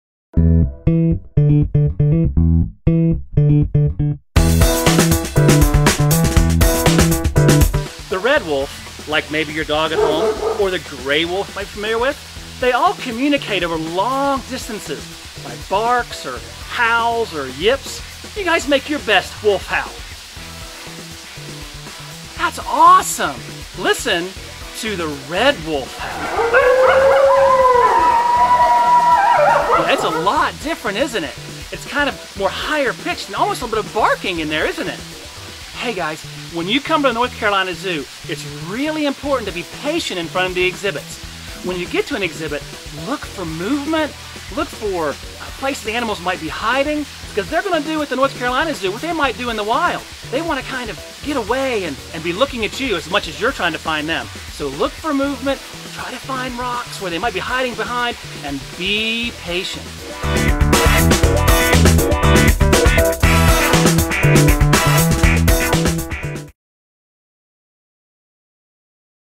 The Red Wolf at the North Carolina Zoo
Go ahead, give a howl! Safari Steve at the North Carolina Zoo takes you inside the Red Wolf habitat and gives you some advice on spotting the animals when you visit the zoo!